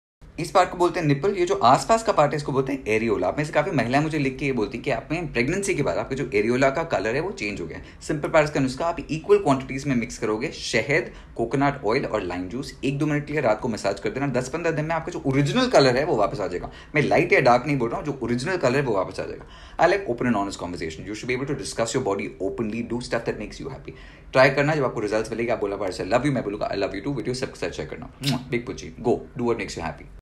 Nipple discolouration
Most requested ParasKeNuskhe especially after your pregnancy .
For more follow on Instagram
I love that you guys ask me these questions openly and comfortably . When you’re doing treatments of any sort for your body , make sure you do it for yourself and not someone else ! share ❤️